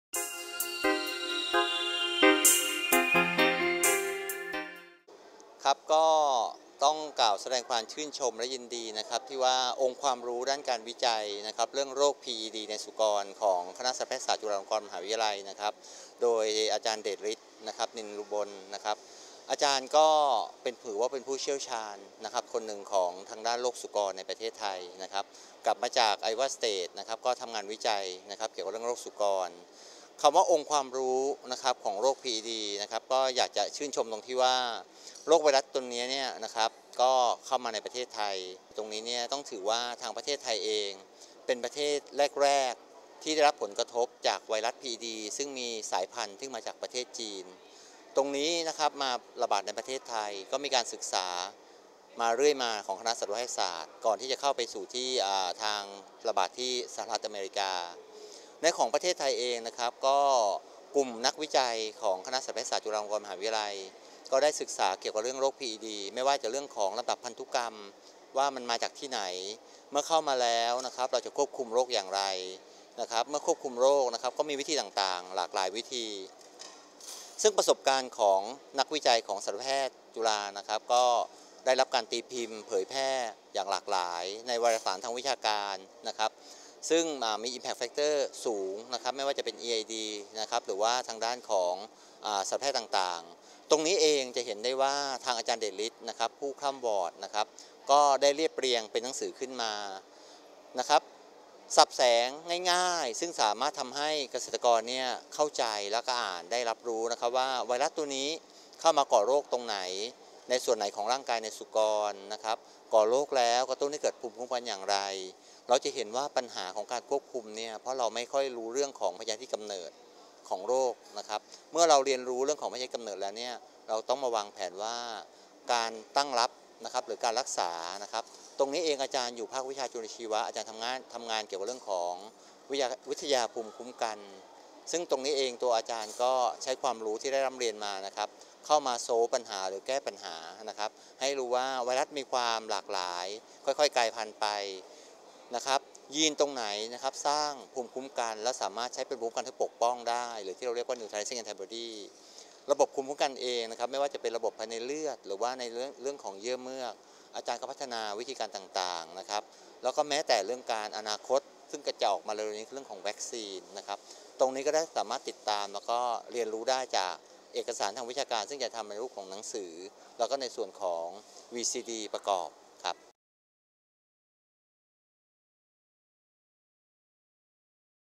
Agri TV : ศ.น.สพ.ดร.รุ่งโรจน์ พูดถึงโรคพีอีดี งานวิจัย ผศ.น.สพ.ดร.เดชฤทธิ์ คณะสัตวแพทย์ฯ จุฬาฯ
Agri TV : ศ.น.สพ.ดร.รุ่งโรจน์ ธนาวงษ์นุเวช คณบดี คณะสัตวแพทย์ศาสตร์ จุฬาลงกรณ์มหาวิทยาลัย กล่าวถึง ผลงานวิจัยและผลงานในเรื่องของ โรคพีอีดีในสุกร ของ ผศ.น.สพ.ดร.เดชฤทธิ์ นิลอุบล  ภาควิชาจุลชีววิทยา คณะสัตวแพทย์ศาสตร์ จุฬาลงกรณ์มหาวิทยาลัย กับโครงการจัดทำคู่มือและวีดีโอเรื่องของโรคพีอีดี ดำเนินการจัดทำโดย พิก แอนด์ พอร์ค และ อะกรี พลัส.